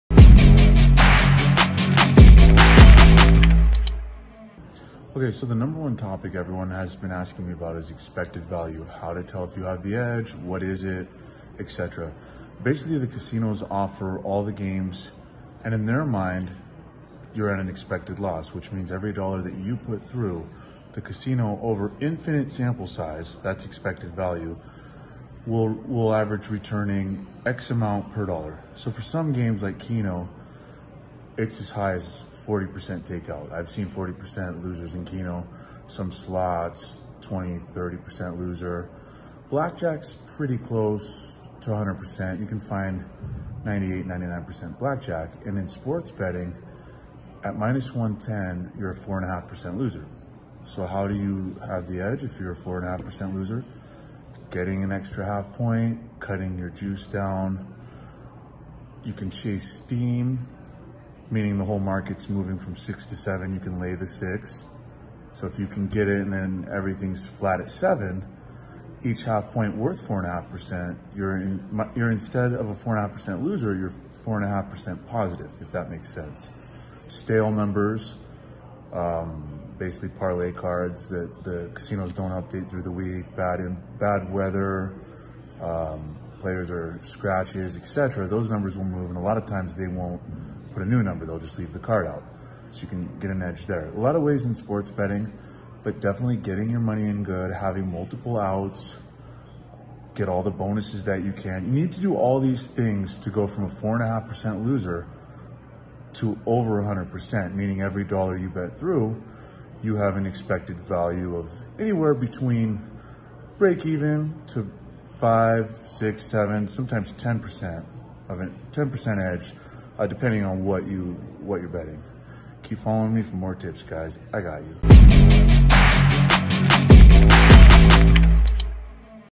+EV Explained in Sports Betting - Expected Value in Sports Betting.
Going over why expected value is important when betting sports.